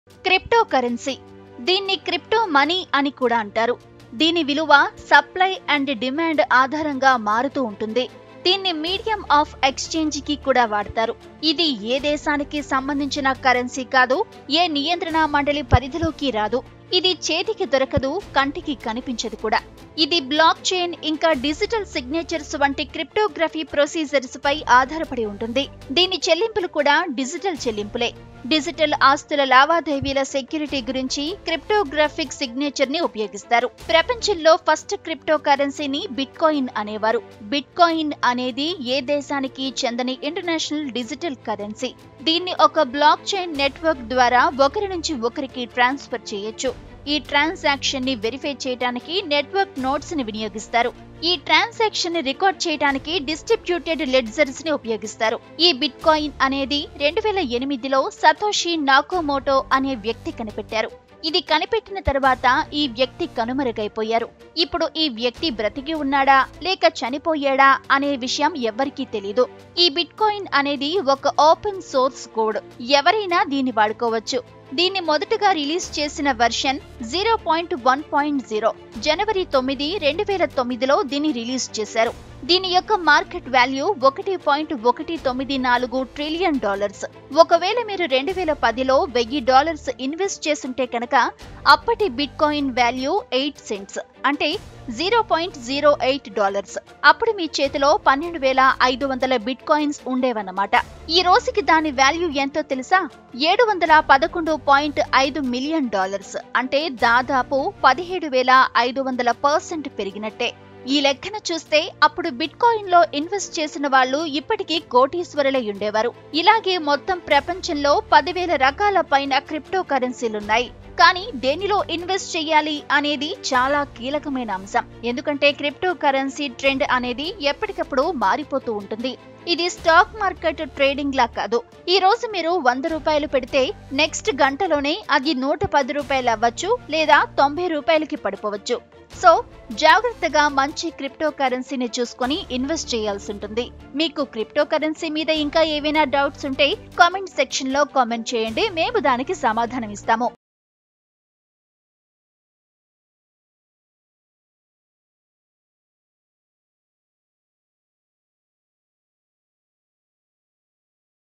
Cryptocurrency : Crypto.. Crypto.. Crypto What Is It ? || Oneindia Telugu
What is Cryptocurrency and what is Bitcoin ? Here we explained.

క్రిప్టో కరెన్సీ.. దీన్ని క్రిప్టో మనీ అని కూడా అంటారు. దీని విలువ డిమాండ్ అండ్ సప్ప్లై ఆధారంగా మారుతుంది. దీన్ని మీడియం ఆఫ్ ఎక్స్చేంజి కి కూడా వాడతారు. ఇది ఏ దేశానికీ సంబంధించిన కరెన్సీ కాదు ఏ నియంత్రణ మండలి పరిధిలోకి రాదు. ఇది చేతికి దొరకదు, కంటికి కనిపించదు. దీన్ని బ్లాక్ చైన్ ఇంకా డిజిటల్ సిగ్నచర్స్ వంటి క్రిప్టోగ్రాఫిక్ ప్రొసీజర్స్ పై ఆధారపడి ఉంటుంది.